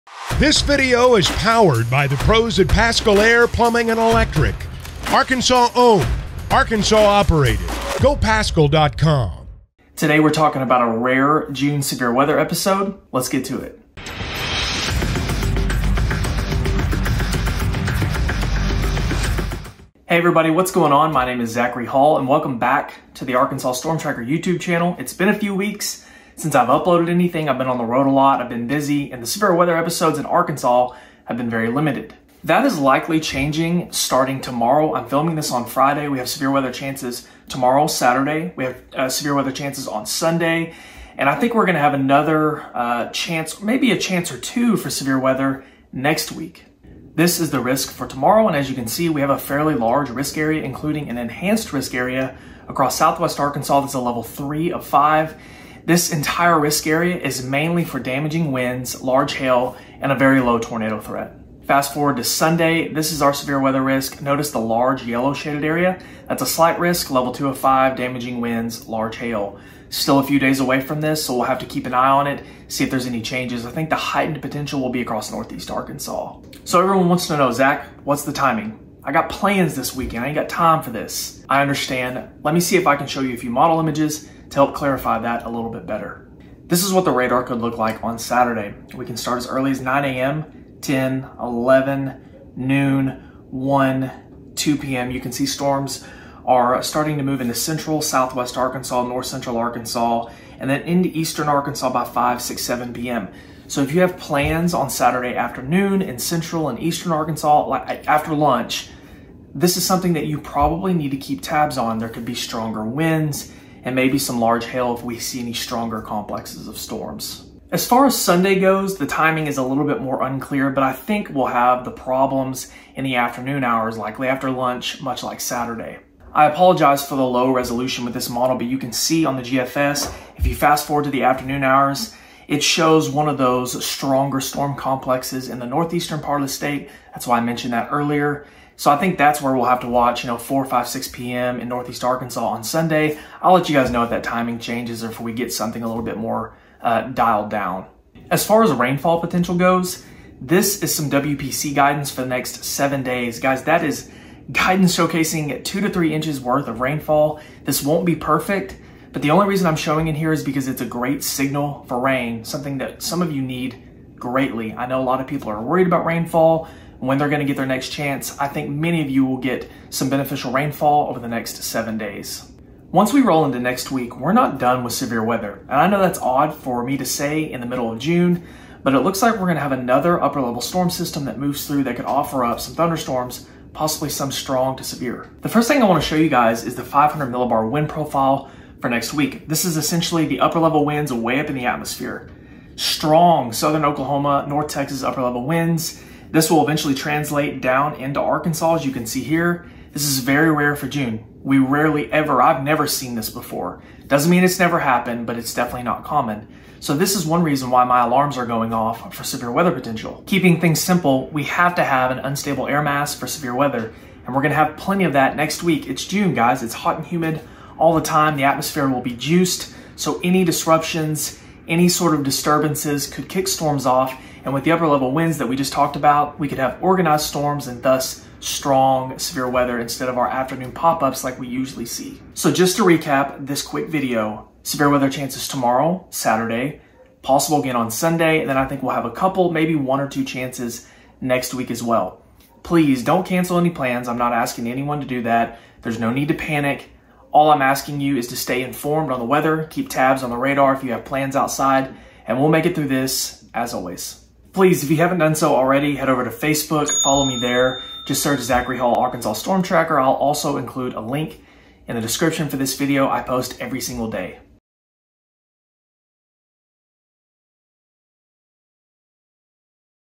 Severe Storms Possible...
It looks likely that some of you could see some strong/severe thunderstorms over the next couple of days. We might even see more next week, but that is still up for discussion.

Join me as I discuss the latest data and go over the details for the next few days.